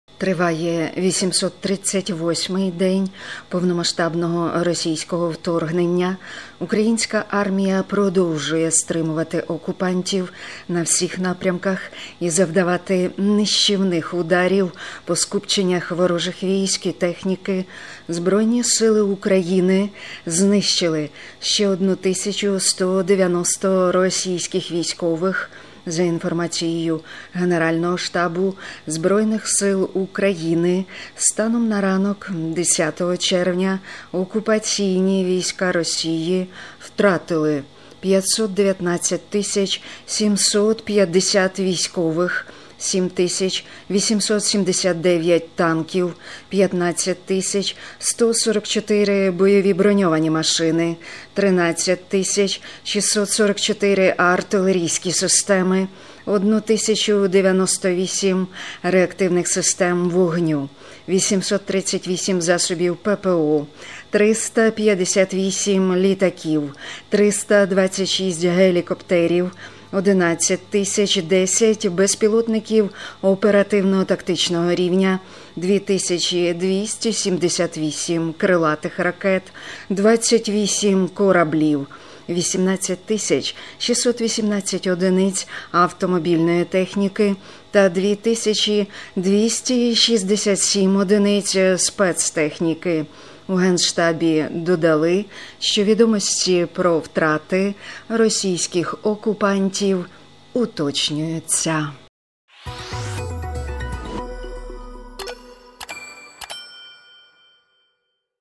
☠️💣Генштаб ЗСУ: втрати Росії в Україні станом на 10 червня
Триває 838 день повномасштабного російського вторгнення.
Українська армія продовжує стримувати окупантів на всіх напрямках і завдавати нищівних ударів по скупченнях ворожих військ та техніки. ЗСУ знищили ще 1 190 російських військових.
За інформацією Генерального штабу Збройних сил України, станом на ранок 10 червня окупаційні війська Росії втратили 519 750 військових, 7 879 танків,
15 144 бойові броньовані машини, 13 644 артилерійські системи, 1 098 реактивних систем залпового вогню, 837 засобів ППО, 358 літаків, 326 гелікоптерів, 11 010 безпілотників оперативно- тактичного рівня, 2 278 крилатих
ракет, 28 кораблів, 18 618 одиниць автомобільної техніки та 2 267 одиниць спецтехніки.
У Генштабі додали, що відомості про втрати російських окупантів уточнюються.